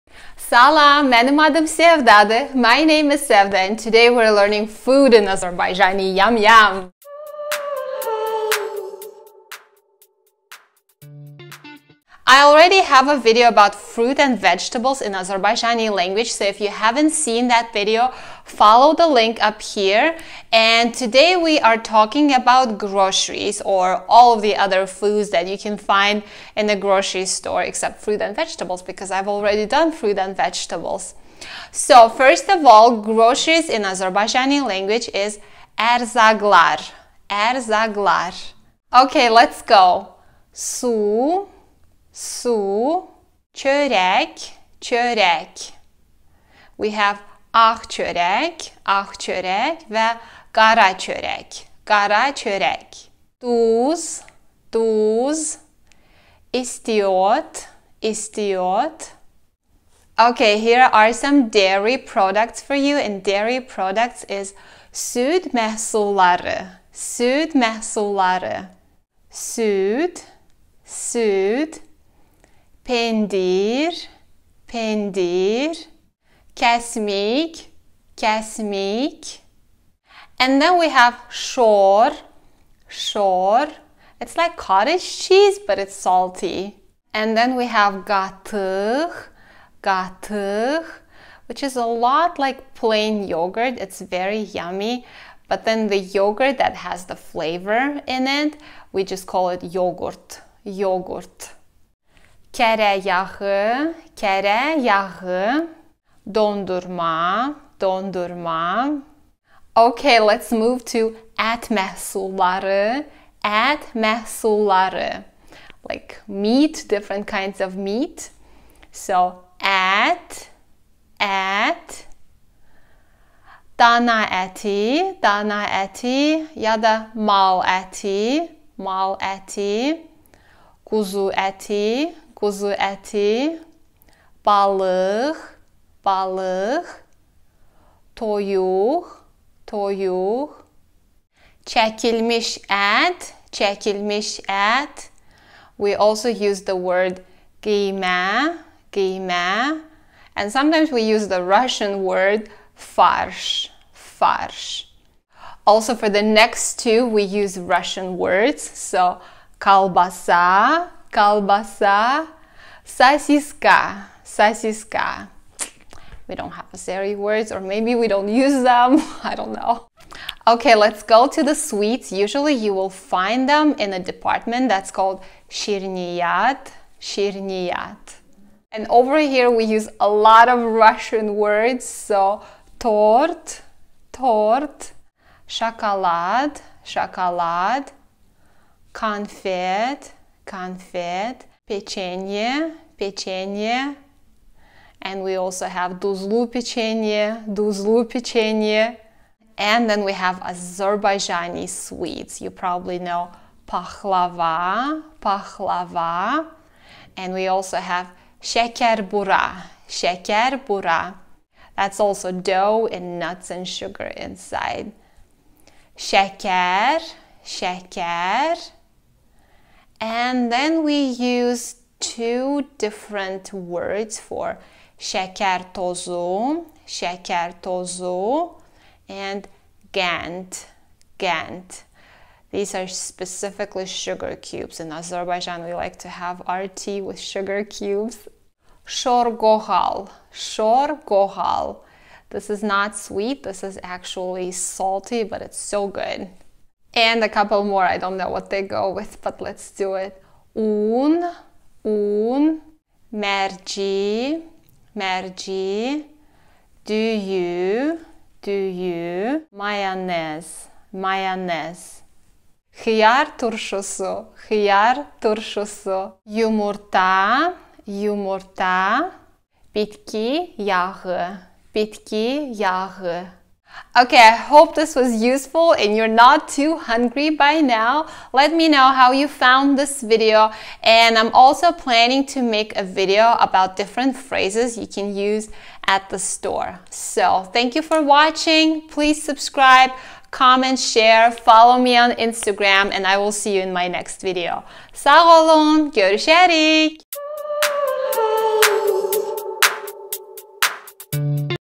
Learn Azerbaijani: Food/Groceries. Lesson 10
Learn the names of common groceries in Azerbaijani!

My Instagram: Learn Azerbaijani with Sevda